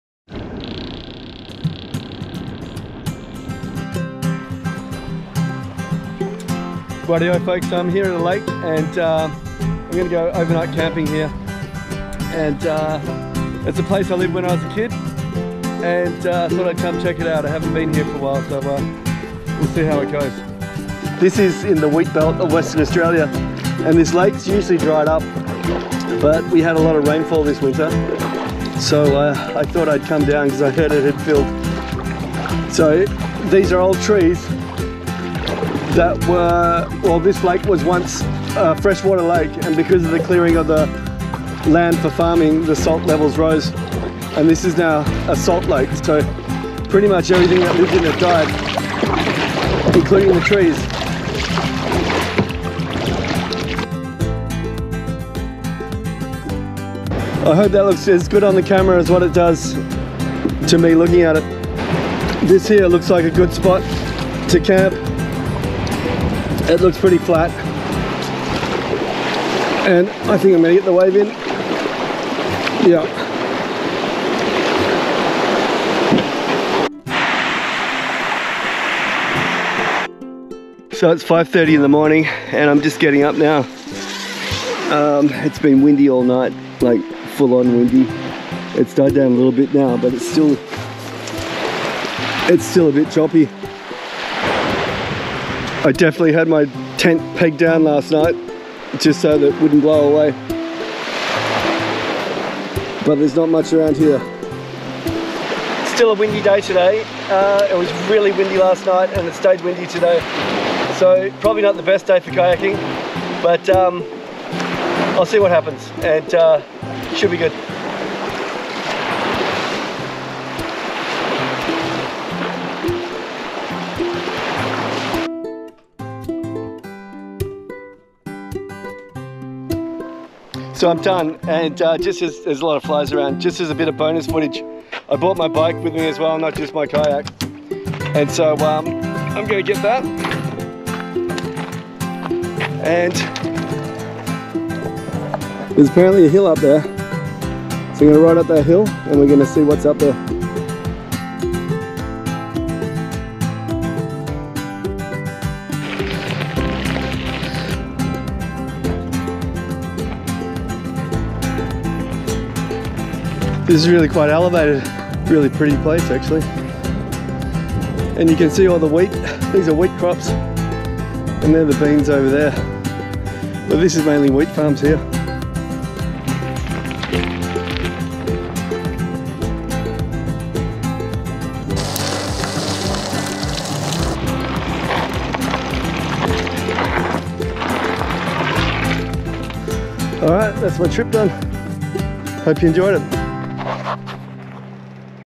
Kayak Camping solo overnight
Solo Kayak lake camping with MTB mountain bike run in remote Wheatbelt WA Lake Dumbleyung